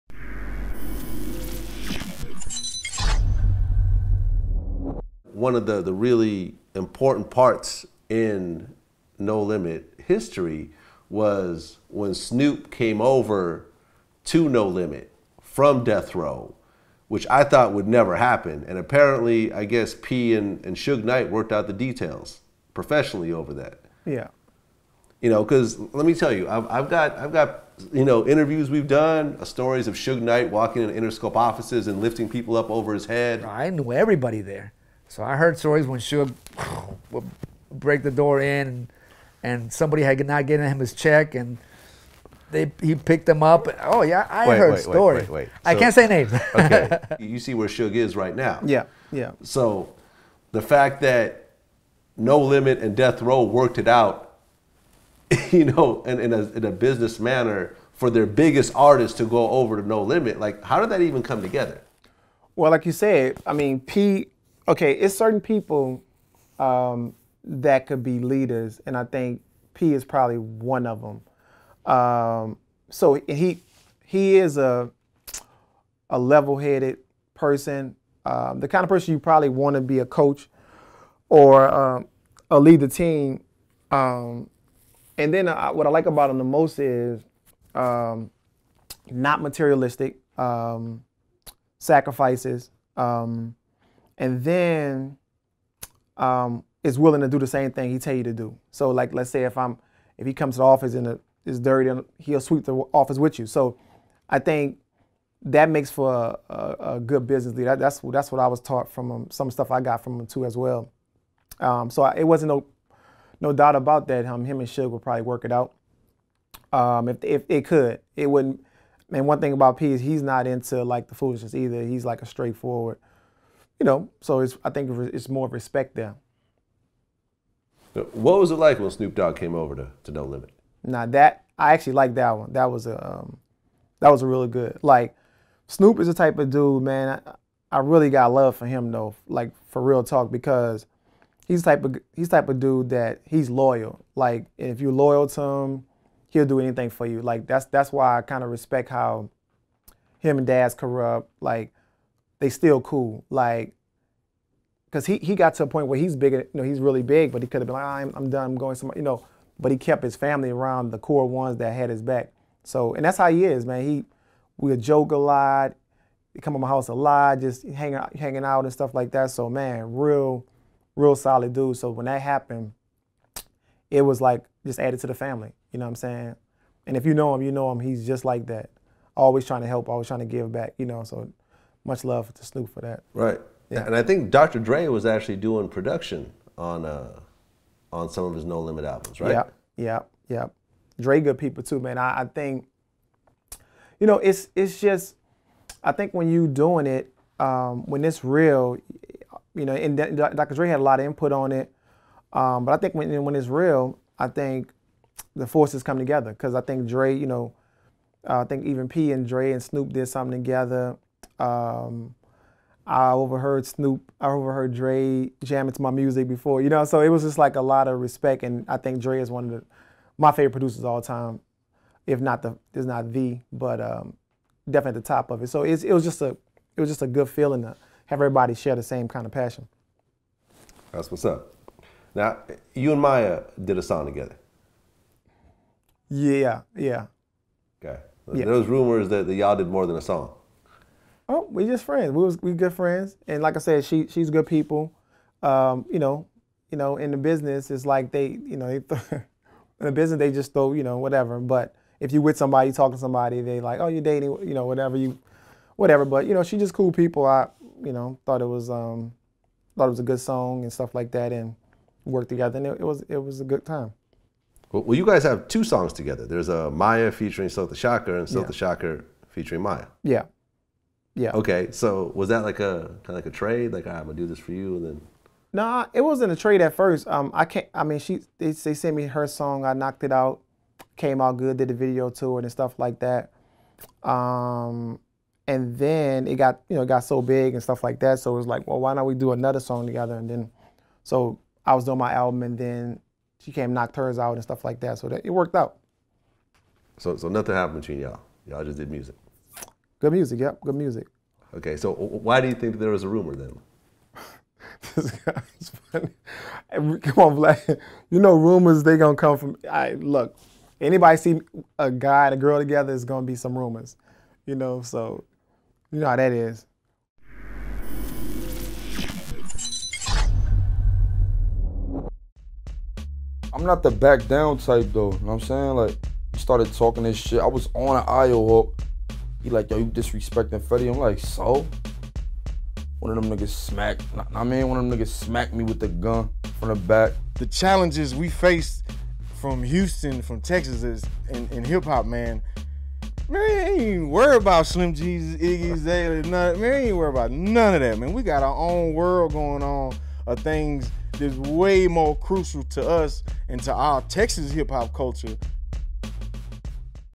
Silkk The Shocker on Master P Signing Away Snoop Dogg from Suge Knight
- "It's certain people that could be leaders, and I think P is one of them." Slikk The Shocker explained about his brother, Master P. "He is a level-headed person, the kind of person you probably want to be a coach or lead the team."

During an interview with DJ Vlad, the New Orleans native explained how Snoop Dogg was able to leave the strong grips of Suge Knight at Death Row, and eventually sign to No Limit Records. While many questioned how the deal went down—due to many rumors of Knights' temper flaring up at any given time— Silkk says the men worked out the business deal with no real issues.

"It was no doubt about that, [that] him and Suge would probably work it out." He told DJ Vlad, "One thing about P, he's not into the foolishness either, he's like a straight forward."